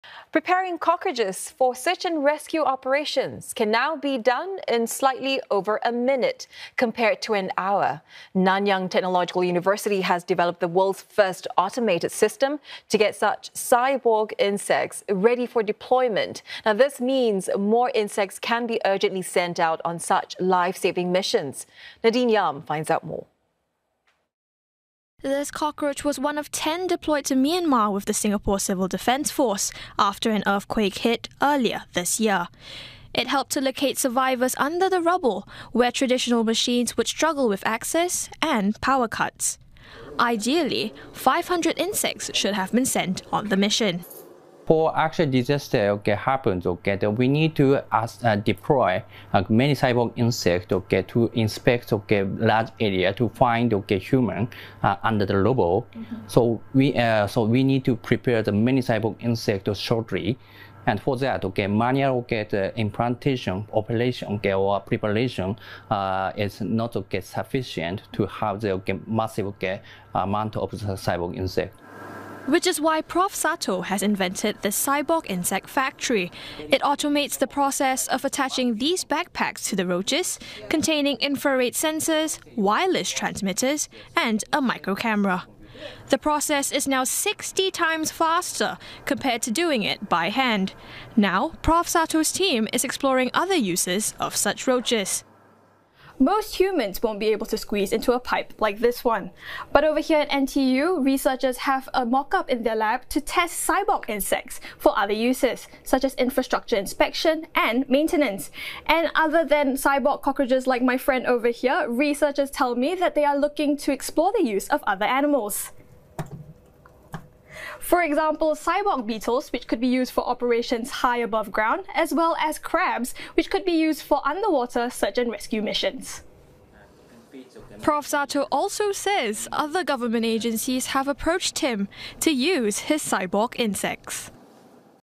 NTU develops world's first automated system to deploy cyborg insects for rescue missions
Preparing cockroaches for search-and-rescue operations can now be done in slightly over a minute, compared to an hour. Nanyang Technological University has developed the world's first automated system to get such cyborg insects ready for deployment. This means more insects can be urgently sent out on life-saving missions. Nadine Yeam reports.